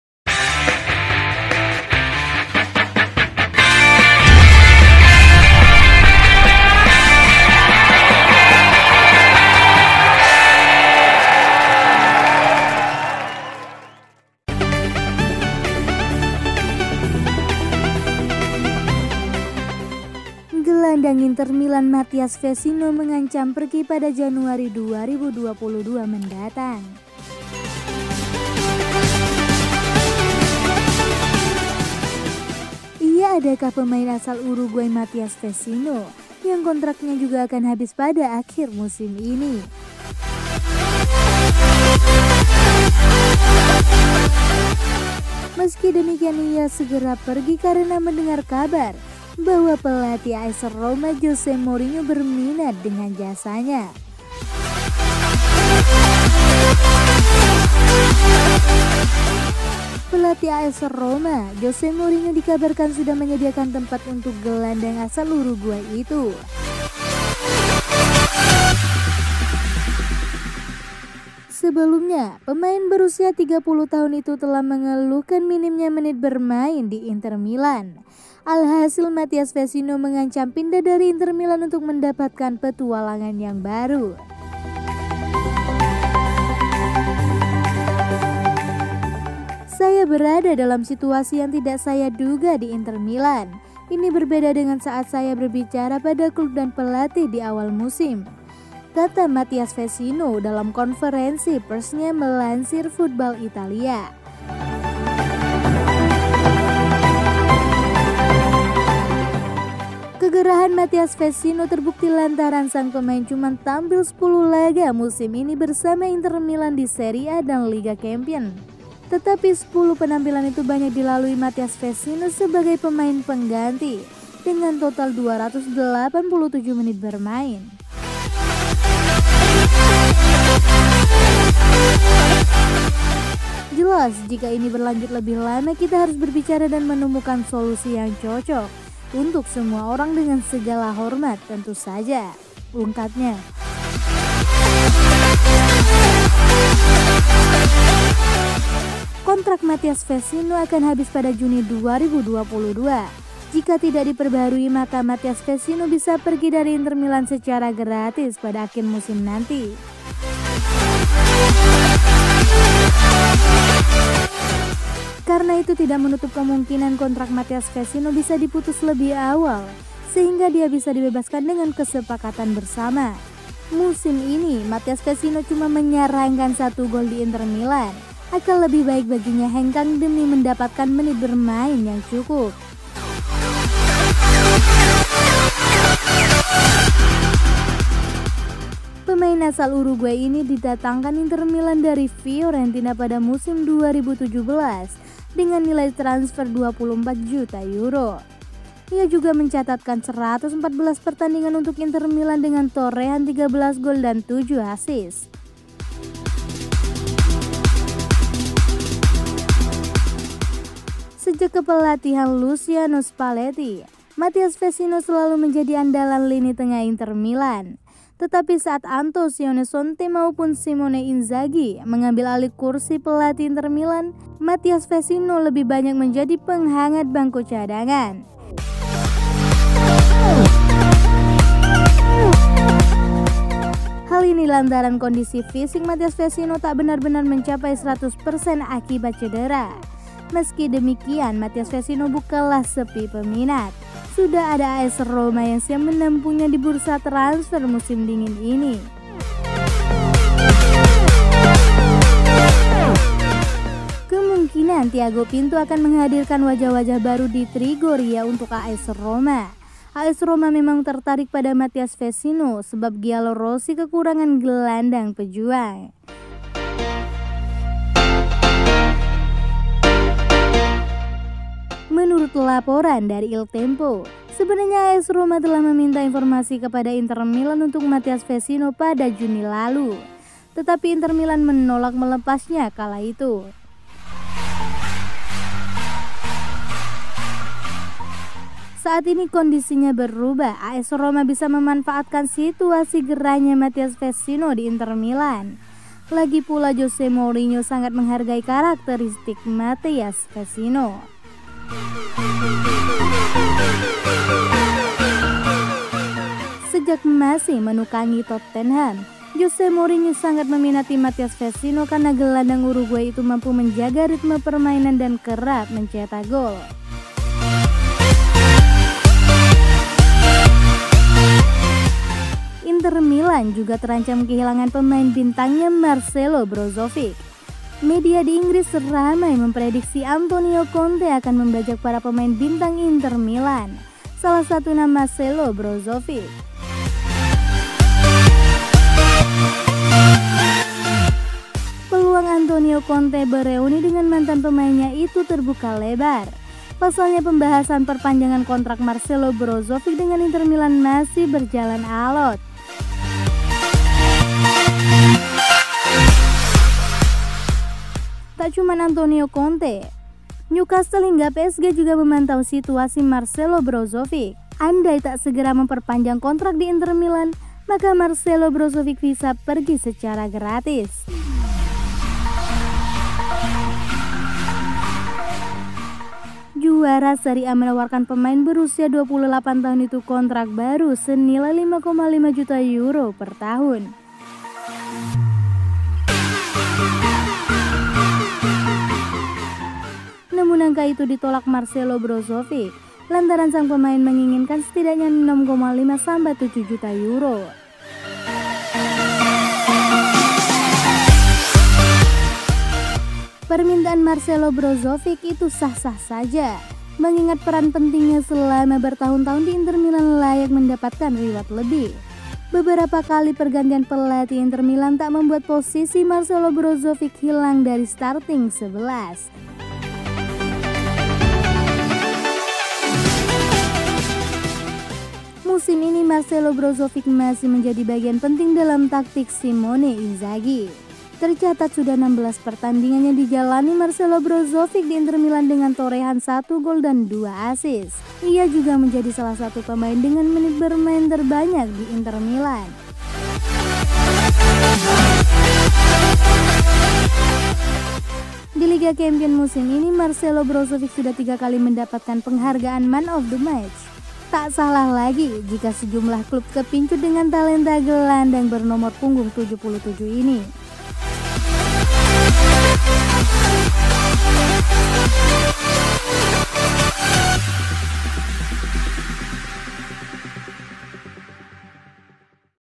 Diam-diam Dapatkan Pemain Baru😱 Mourinho Manfaatkan Situasi Panas Sang Rival🔥Amunisi baru 😲
Dibujuk Pelatih AS Roma Jose Mourinho, Gelandang Inter Milan asal Uruguay Minta Dijual Januari Ini

Gelandang Inter Milan Matias Vecino mengancam pergi pada Januari 2022 mendatang.

Ia adakah pemain asal Uruguay Matias Vecino yang kontraknya juga akan habis pada akhir musim ini.

Meski demikian ia ingin segera pergi karena mendengar kabar bahwa pelatih AS Roma Jose Mourinho berminat dengan jasanya..

Pelatih AS Roma, Jose Mourinho dikabarkan sudah menyediakan tempat untuk gelandang asal Uruguay itu.

Sebelumnya, pemain berusia 30 tahun itu telah mengeluhkan minimnya menit bermain di Inter Milan.

Alhasil, Matias Vecino mengancam pindah dari Inter Milan untuk mendapatkan petualangan baru.

"Saya berada dalam situasi yang tidak saya duga di Inter Milan."

"Ini berbeda dengan saat saya berbicara pada klub dan pelatih di awal musim,” kata Matias Vecino dalam konferensi persnya, melansir Football Italia.

Kegerahan Matias Vecino terbukti lantaran sang pemain cuma tampil 10 laga musim ini bersama Inter Milan di Serie A dan Liga Champions.

Tetapi 10 penampilan itu banyak dilalui Matias Vecino sebagai pemain pengganti, dengan total 287 menit bermain.

"Jelas, jika ini berlanjut lebih lama, kita harus berbicara dan menemukan solusi yang cocok untuk semua orang. Dengan segala hormat, tentu saja," ungkapnya.

Kontrak Matias Vecino akan habis pada Juni 2022.

Jika tidak diperbaharui, maka Matias Vecino bisa pergi dari Inter Milan secara gratis pada akhir musim nanti.

Karena itu, tidak menutup kemungkinan kontrak Matias Vecino bisa diputus lebih awal sehingga dia bisa dibebaskan dengan kesepakatan bersama.

Musim ini, Matias Vecino cuma menyarangkan satu gol di Inter Milan, akan lebih baik baginya hengkang demi mendapatkan menit bermain yang cukup.

Pemain asal Uruguay ini didatangkan Inter Milan dari Fiorentina pada musim 2017 dengan nilai transfer 24 juta euro.

Ia sudah mencatatkan 114 pertandingan untuk Inter Milan dengan torehan 13 gol dan 7 assist.

Sejak kepelatihan Luciano Spaletti, Matias Vecino selalu menjadi andalan lini tengah Inter Milan.

Tetapi saat Antonio Conte maupun Simone Inzaghi mengambil alih kursi pelatih Inter Milan, Matias Vecino lebih banyak menjadi penghangat bangku cadangan.

Hal ini lantaran kondisi fisik Matias Vecino tak benar-benar mencapai 100 persen akibat cedera.

Meski demikian, Matias Vecino bukanlah sepi peminat.

Sudah ada AS Roma yang siap menampungnya di bursa transfer musim dingin ini.

Kemungkinan Tiago Pinto akan menghadirkan wajah-wajah baru di Trigoria untuk AS Roma.

AS Roma memang tertarik pada Matias Vecino, sebab Gialorossi kekurangan gelandang pejuang.

Menurut laporan dari Il Tempo, sebenarnya AS Roma telah meminta informasi kepada Inter Milan untuk Matias Vecino pada Juni lalu.

Tetapi Inter Milan menolak melepasnya kala itu.

Saat ini, kondisinya berubah, AS Roma bisa memanfaatkan situasi gerahnya Matias Vecino di Inter Milan.

Lagipula, Jose Mourinho sangat menghargai karakteristik Matias Vecino.

Sejak masih menukangi Tottenham, Jose Mourinho sangat meminati Matias Vecino karena gelandang Uruguay itu mampu menjaga ritme permainan dan kerap mencetak gol.

Inter Milan juga terancam kehilangan pemain bintangnya, Marcelo Brozovic.

Media di Inggris ramai memprediksi Antonio Conte akan membajak para pemain bintang Inter Milan, salah satunya Marcelo Brozovic.

Peluang Antonio Conte bereuni dengan mantan pemainnya itu terbuka lebar.

Pasalnya pembahasan perpanjangan kontrak Marcelo Brozovic dengan Inter Milan masih berjalan alot.

Tak cuma Antonio Conte, Newcastel hingga PSG juga memantau situasi Marcelo Brozovic.

Andai tak segera memperpanjang kontrak di Inter Milan, maka Marcelo Brozovic bisa pergi secara gratis.

Juara Serie A menawarkan pemain berusia 28 tahun itu kontrak baru senilai 5,5 juta euro per tahun.

Namun angka itu ditolak Marcelo Brozovic, lantaran sang pemain menginginkan setidaknya 6,5-7 juta euro.

Perimntaan Marccelo Brozovic itu sah-sah saja, mengingat peran pentingnya selama bertahun-tahun di Inter Milan layak mendapatkan reward lebih.

Beberapa kali pergantian pelatih Inter Milan tak membuat posisi Marcelo Brozovic hilang dari starting XI.

Musim ini, Marcelo Brozovic masih menjadi bagian penting dalam taktik Simone Inzaghi.

Tercatat sudah 16 pertandingan yang dijalani Marcelo Brozovic di Inter Milan dengan torehan satu gol dan 2 assists.

Ia juga menjadi salah satu pemain dengan menit bermain terbanyak di Inter Milan.

Di Liga Champions musim ini, Marcelo Brozovic sudah tiga kali mendapat penghargaan Man of The Match.

Tak salah jika sejumlah klub kepincut dengan talenta gelandang bernomor punggung 77 ini.